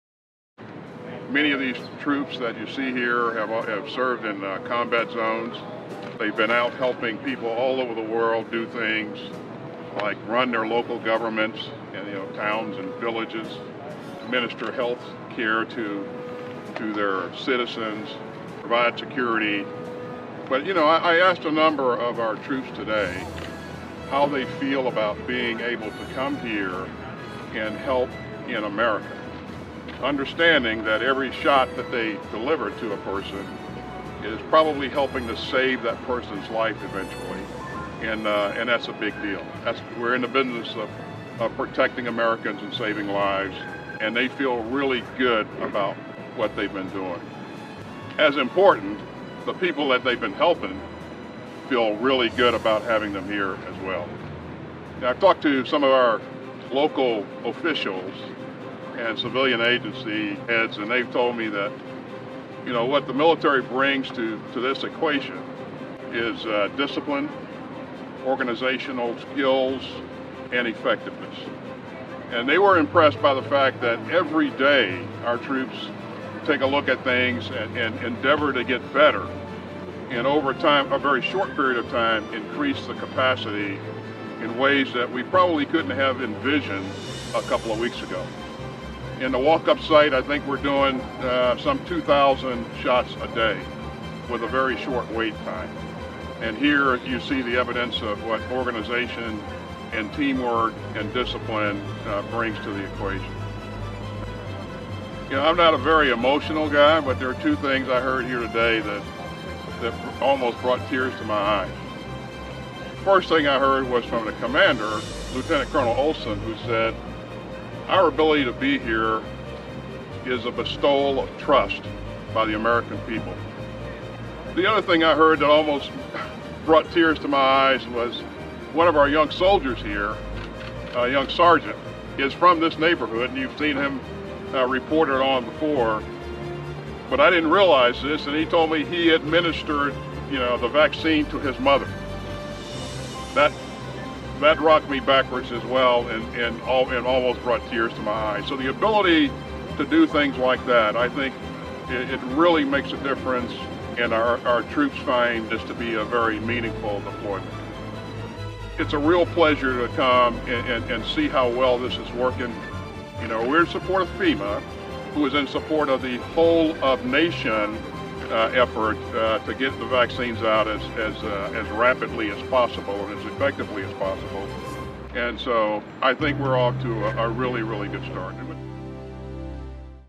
U.S. Defense Secretary Lloyd Austin visits the Cal State LA COVID-19 community vaccination center
Defense Secretary Lloyd Austin visits the COVID-19 community vaccination center at California State University Los Angeles, Feb. 24, 2021. The center is the nation’s first federal response to the pandemic that includes the Federal Emergency Management Agency, the California Governor’s Office of Emergency Services, active-duty military, and the California National Guard. (U.S. Air National Guard video by Senior Airman Neil Mabini)